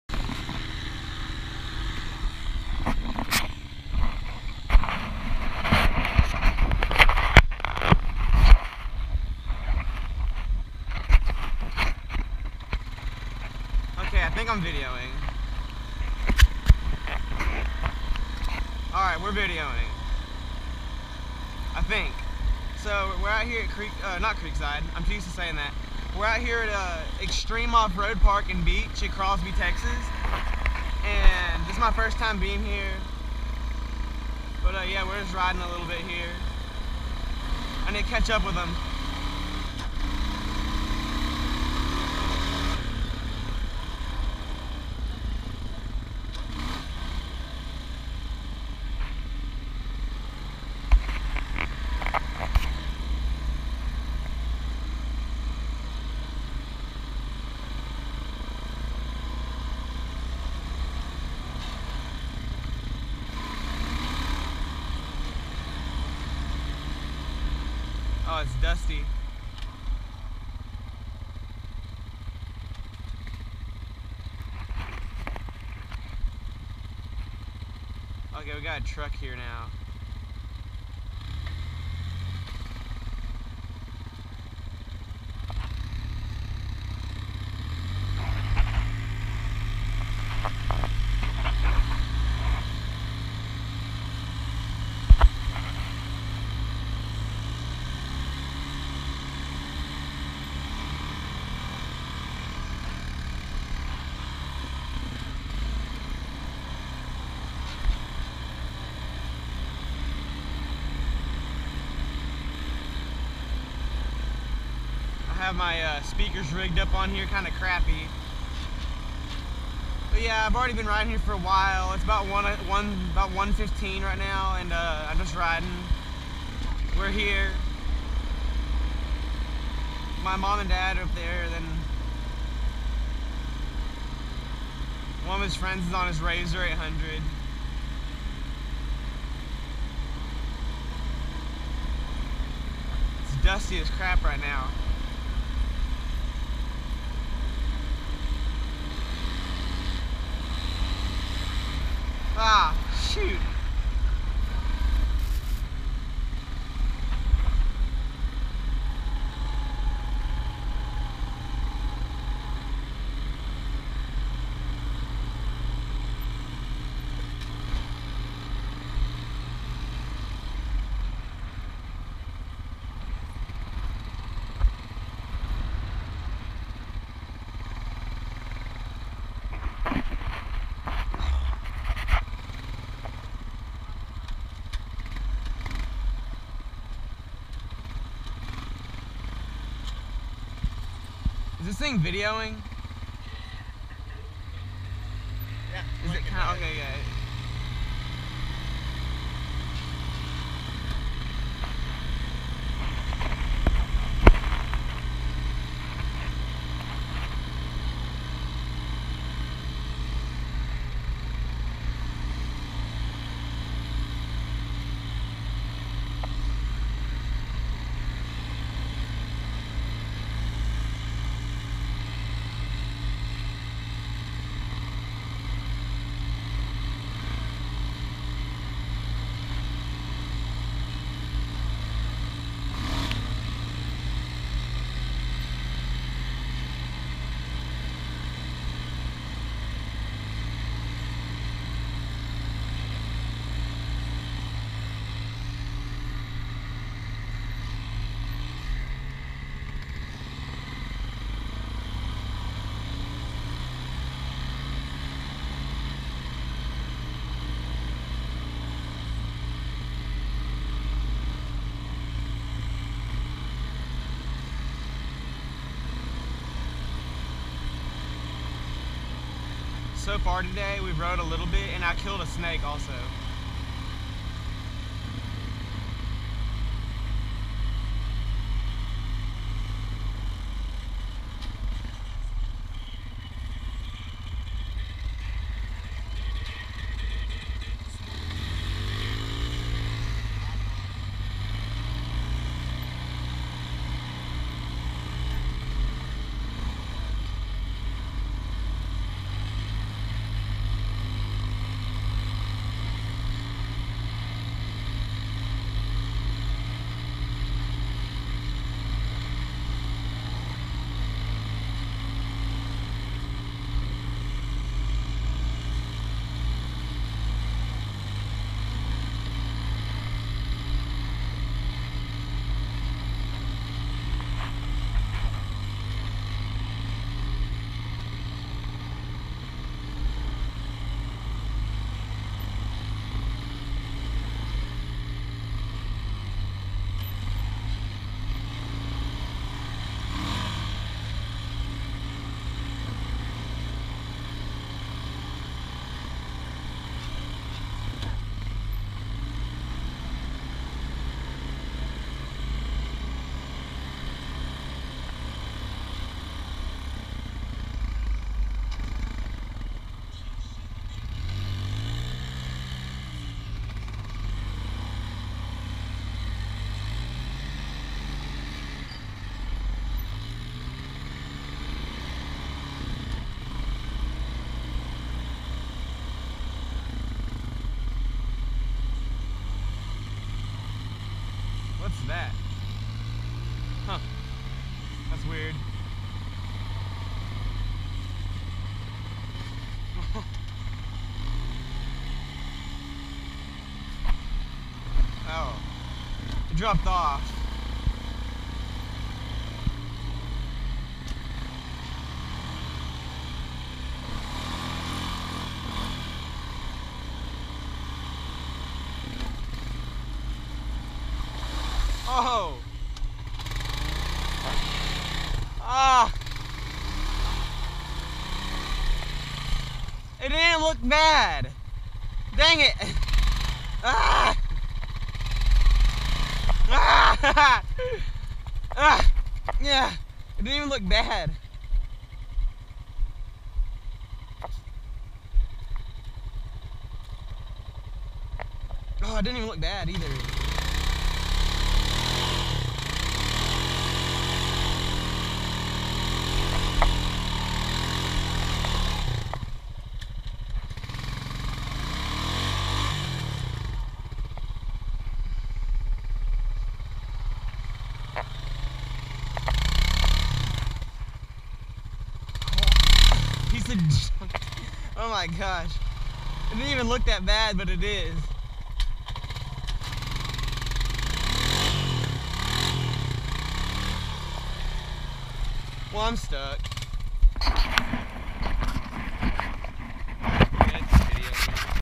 ridding at xtreme offroad park
my only video from xtreme offroad park i am so sorry but we were only there for a few hours so i didnt get but one good video